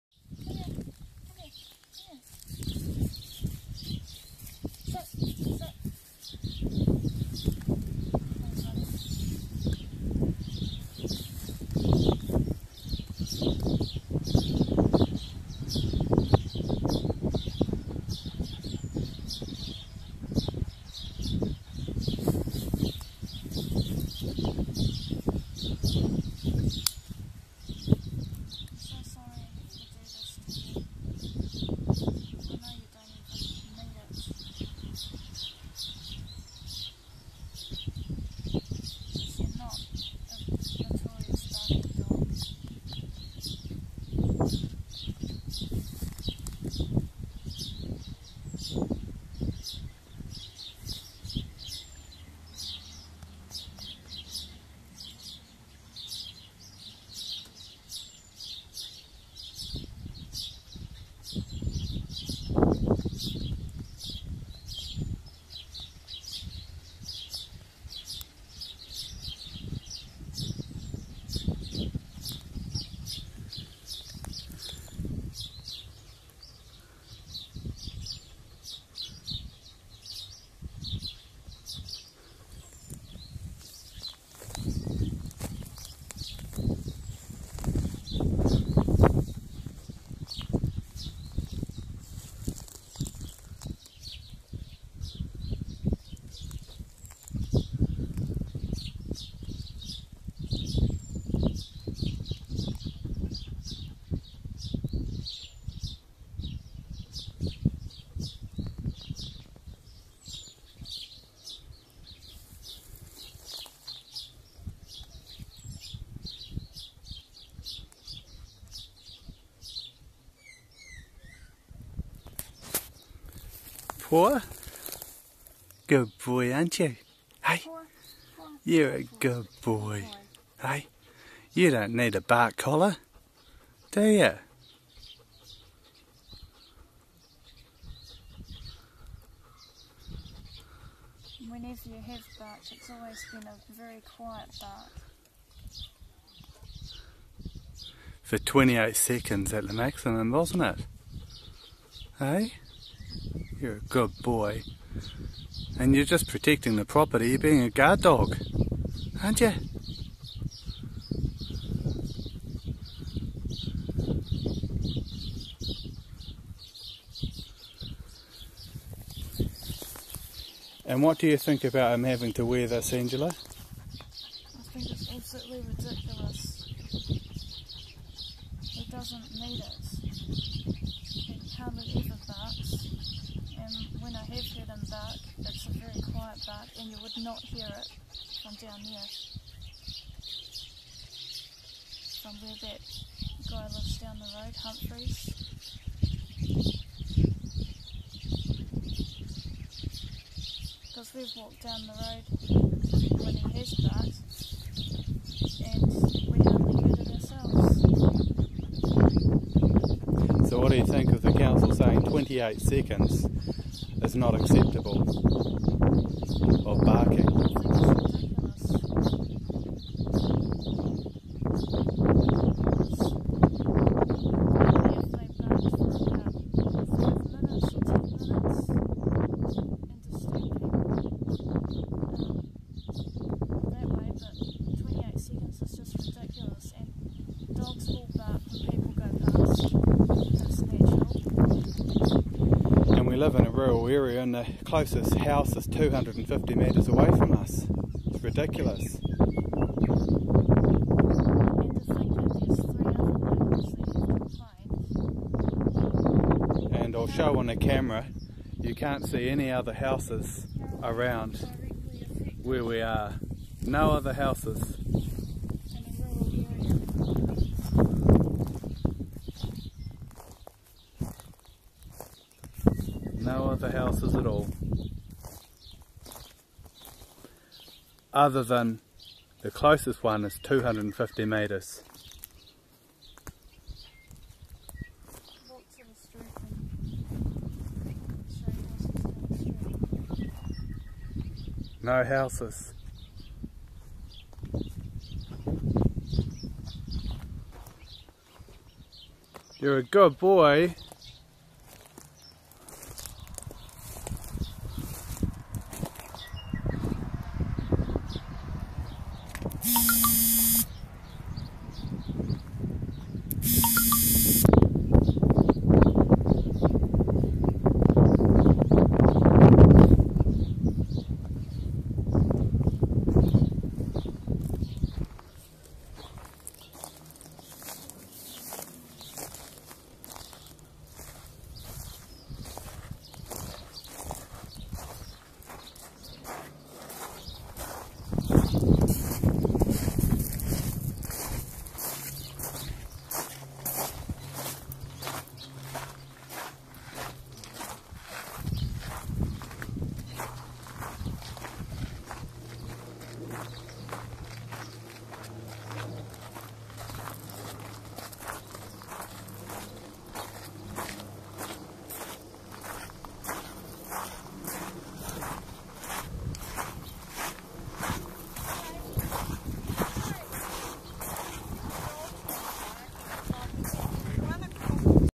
Shock collar shame. Bullied by Southland District Council Staff! NZ
Shock collar. Bullied by southland dc four locals saying it is our dog barking when its not! One of the locals turns up as we are recording utube video. That local lives well over 800metres away. The closest house is 250metres away. There are eight dogs in the same area. Its not possible to see our dog from that distance. We have cctv footage with time and date. Council staff say not intrested IN VEIWING. THE COUNCIL SAYS all four locals will complete a affidavit in support of their complaints. Shame on locals and council staff!! It is not our dog barking!Veiw our follow up video.